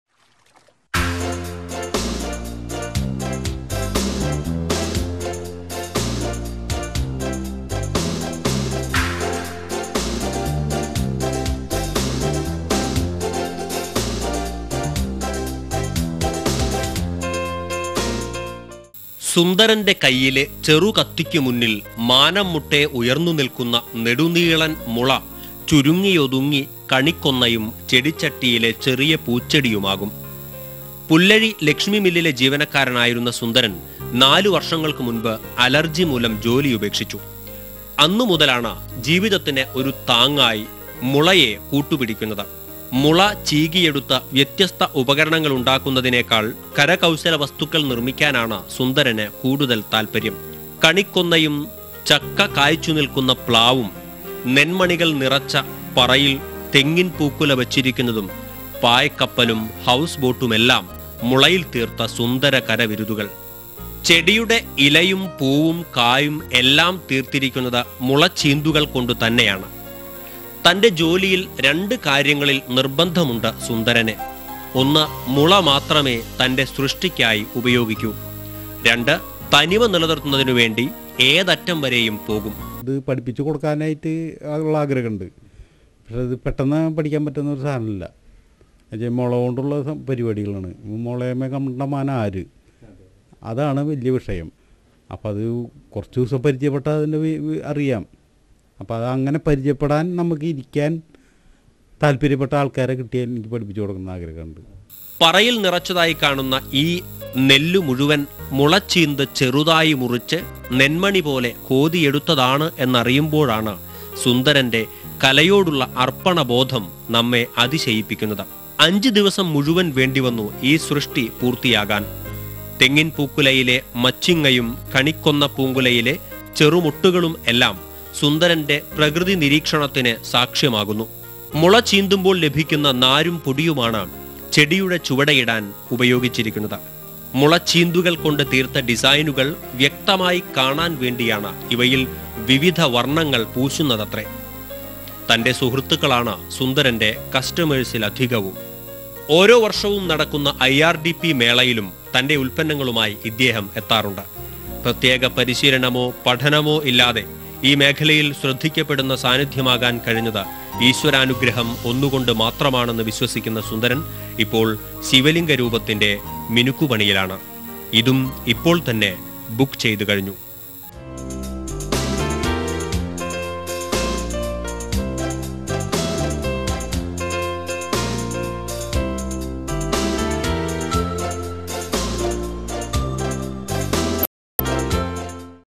Sundaran's Bamboo Arts.DAT
Sundaran is a self made artist working in Bamboos.